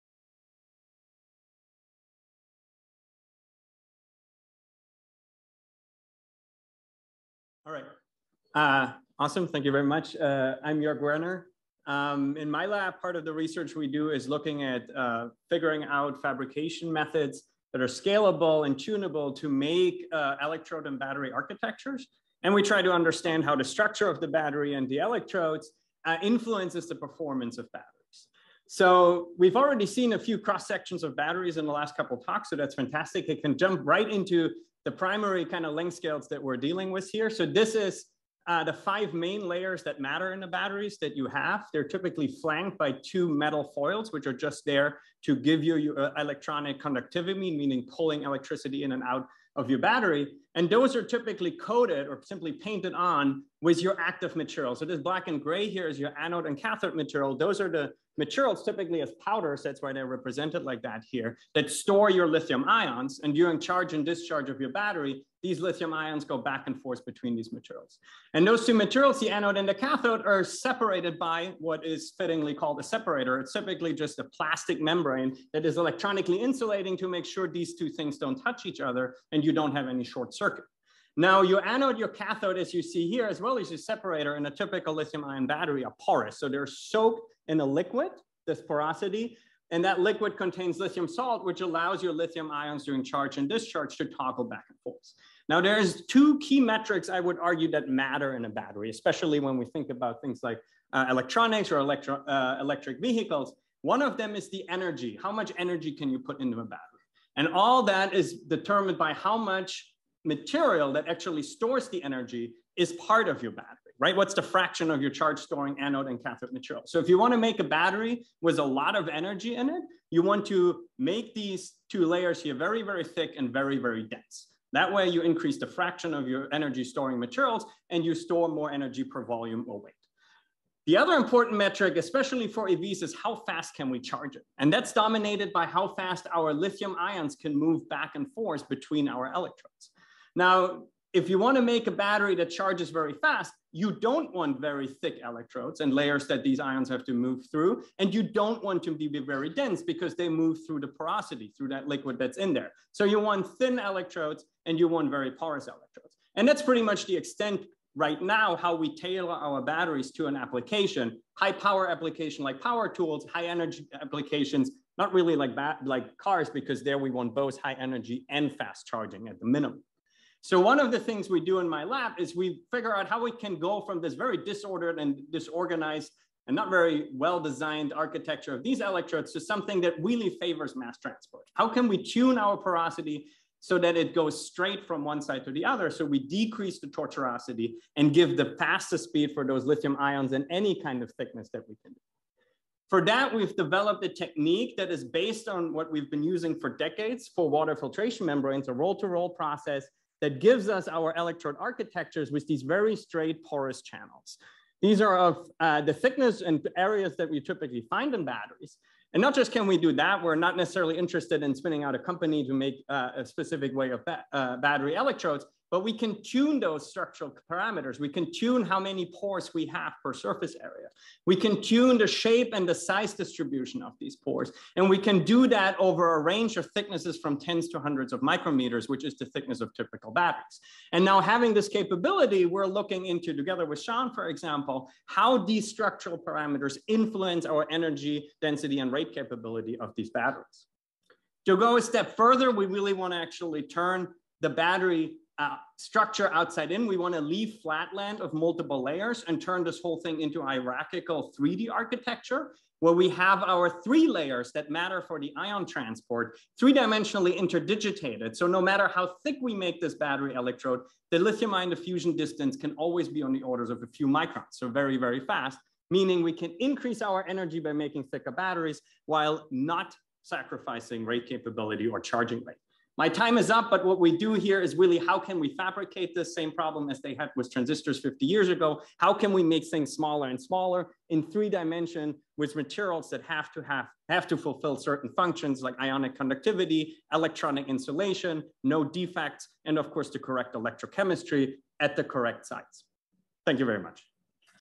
Jorg Werner: Tailored and Architected Batteries
Jorg Werner gives a talk titled “Tailored and Architected Batteries" at BU’s Research on Tap event.